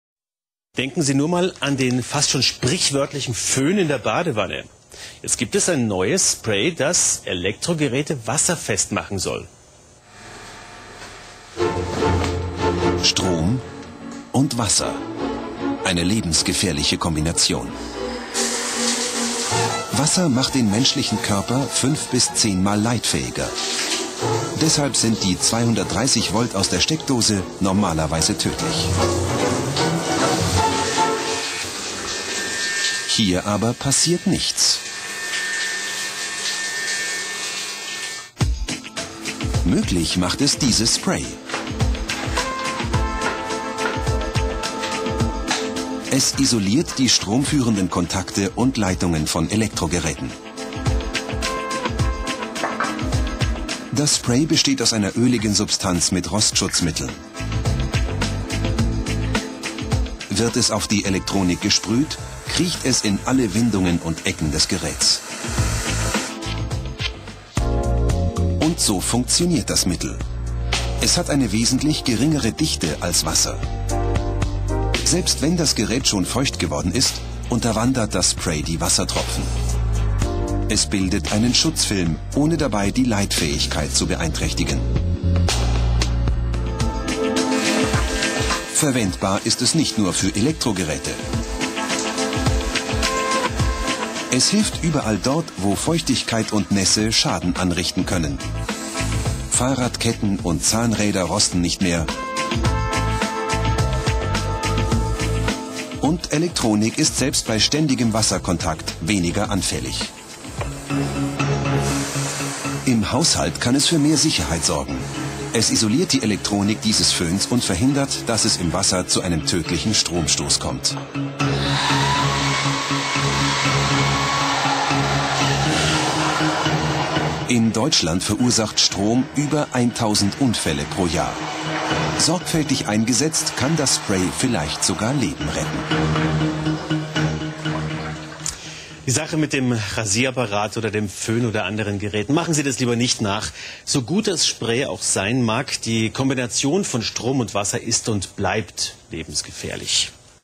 WET PROTECT SPRAY
Sprej, ki zaščiti vse vrste električnih vodnikov. Še posebej priporočljiv za motoriste in nautičarje, katerih prevozna sredstva so izpostavljena vodi.